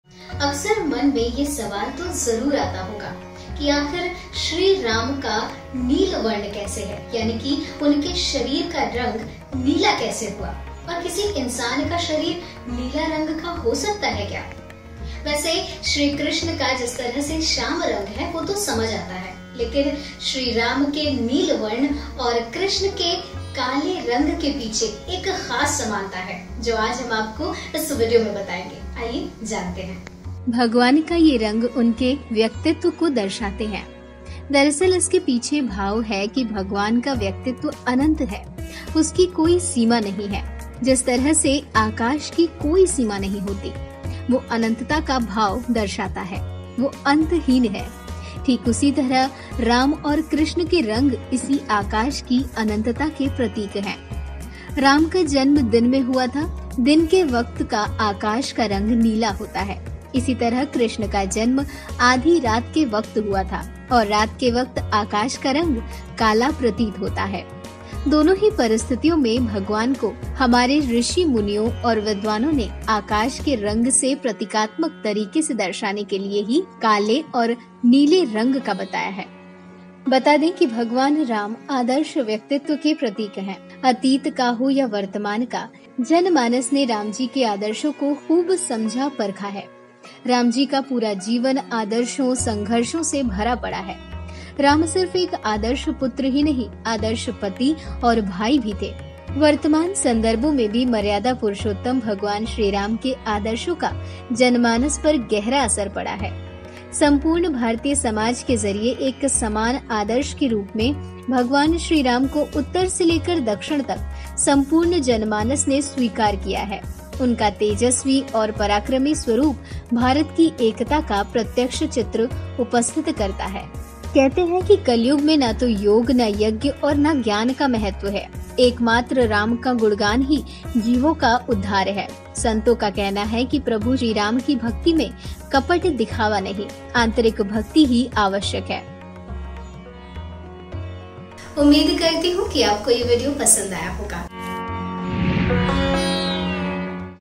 भगवान राम का रंग नीला होने का बड़ा रहस्य | Dharm Ganga
जिन भगवानो  पर आप को इतनी श्रद्धा है, इतना विश्वास है क्या उनके रंग रूप के बारे में कभी आप ने विचारा किया है? वैसे माना जाता है भगवान को किसी ने नहीं देखा है मगर भगवान का जो रंग रूप हमारे धर्म से हमे जानने को मिला है वो ऐसा क्यों है? उनके रंग में इतनी विषमता क्यों है? आइए जानते है क्यों है भगवान राम का रंग नीला  ?

Have you ever thought about the appearance of the Gods on whom you have so much faith, so much faith? By the way, it is believed that no one has seen God, but the colorful form of God that we have got to know from our religion, why is it like this? Why is there such a difference in their color? Let's know why Lord Ram's color is blue?

                ~PR.112~ED.119~HT.178~